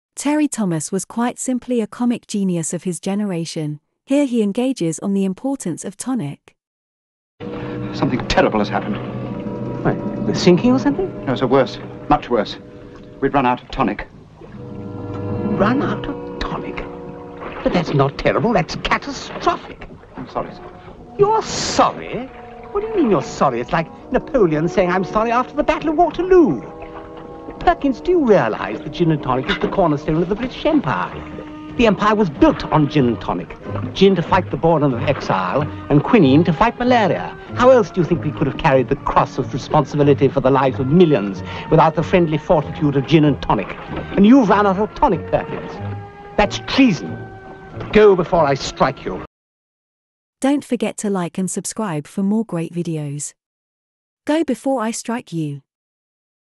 A catastrophe has happened, no tonic!
Terry Thomas the comic genius of his time is appalled to discover that there is a shortage of tonic!! A catastrophe that can not be forgiven.